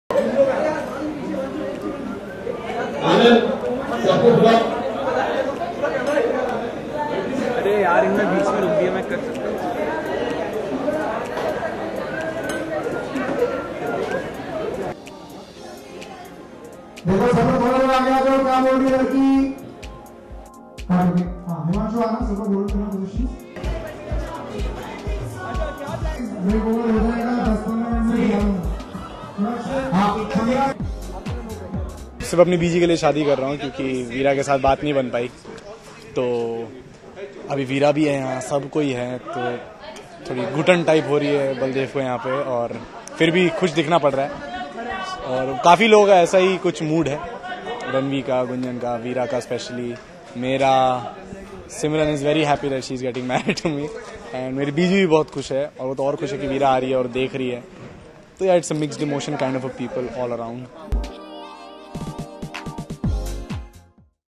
Ek Veer Ki Ardaas Veera Full Episode Shoot | Behind The Scenes | 15th November HD
Watch the exclusive on location footage of the show Ek Veer Ki Ardaas Veera only on Bollywood Firki!